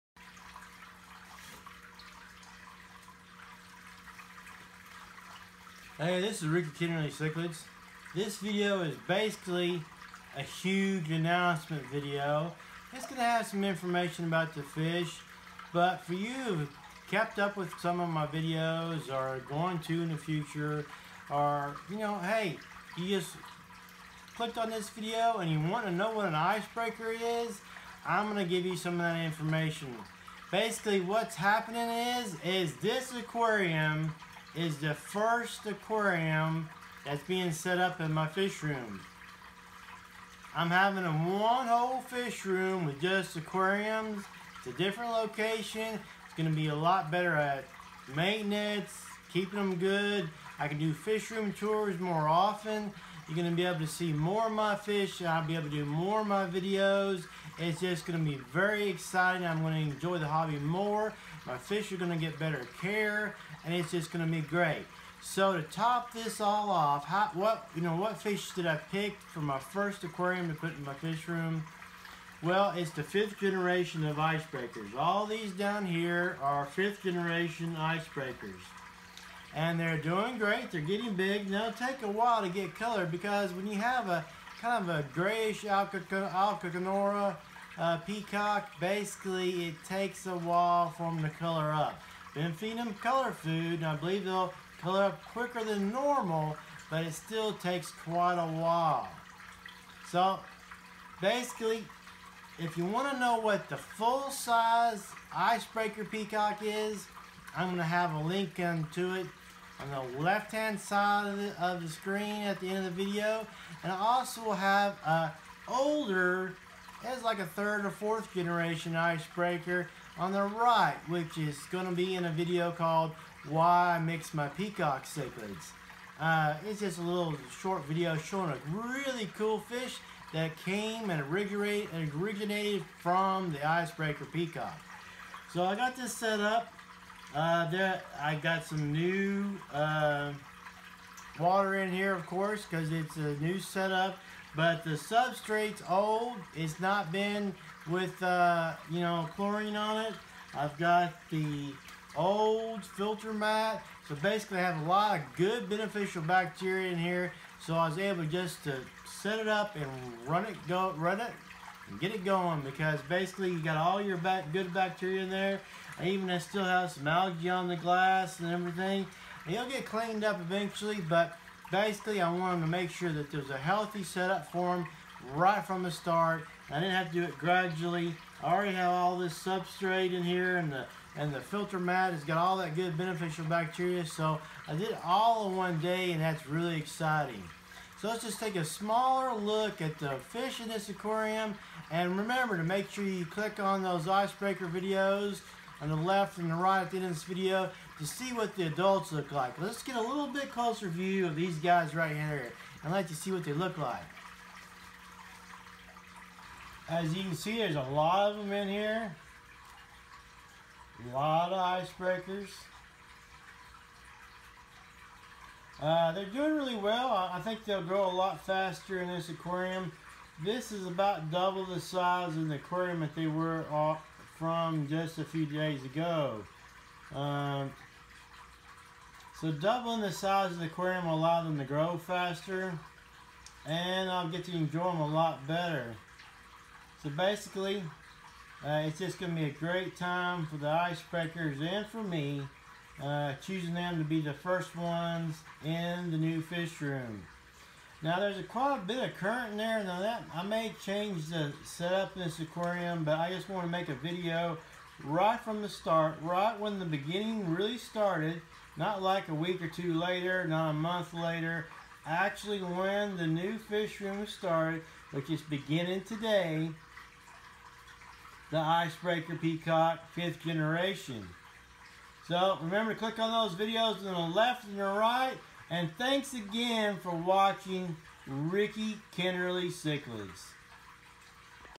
Line Breeding The IceBreaker Peacock Cichlid
This aquarium marks the first aquarium setup in my new fish room.  I picked the Icebreaker Peacock Cichlids to be the first fish in the fish rrom.  It is the starting point of great improvements to my fish keeping channel!